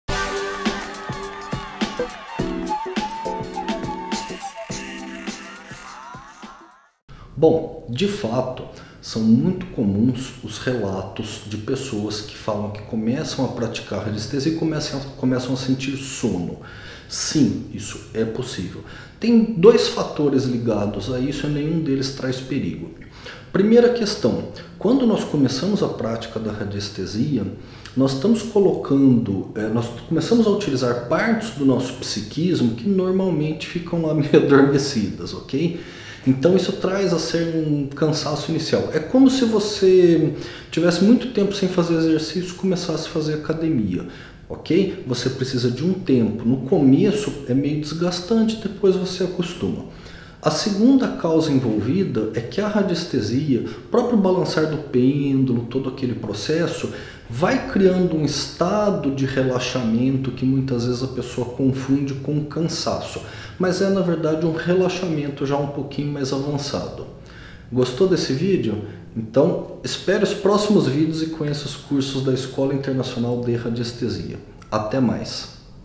Radiestesia dá sono? Isto é perigoso?
Descubra, em 1 minuto, o que é este cansaço que muitas pessoas dizem sentir quando começam praticar radiestesia, e quais as possíveis consequências.